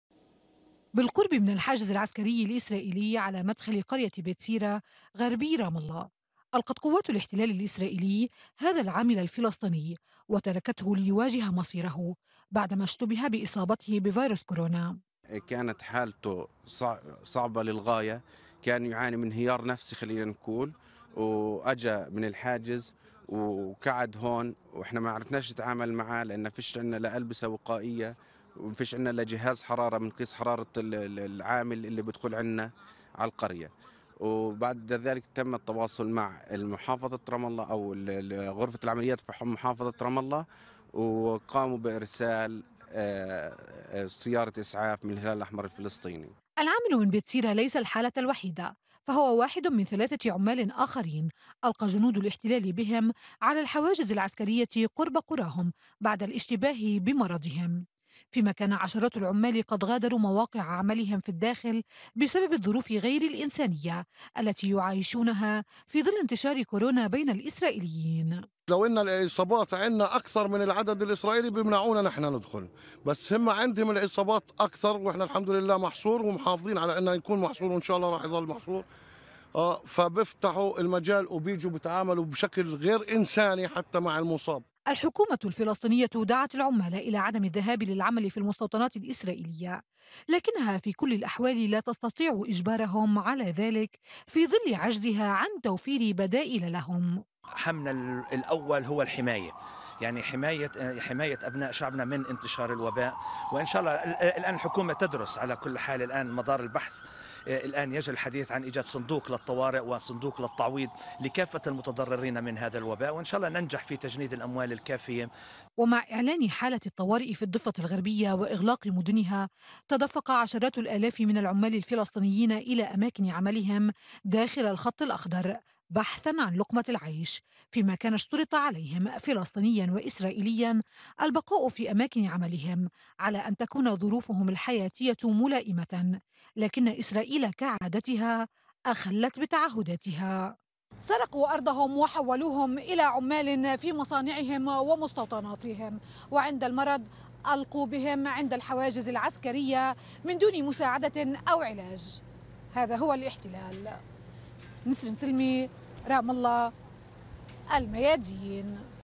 ظروف حياتية صعبة يعيشها العمال الفلسطينيون في الداخل المحتل
إعداد: نسرين سلمي
العمال الفلسطينيون في الداخل المحتل باتوا فريسة السياسات العنصرية وغير الإنسانية للعديد من مشغليهم الإسرائيليين بعد تخلي أرباب العمل عمّن يمرض من العمال، ويجري إلقاؤهم قرب الحواجز العسكرية لقراهم.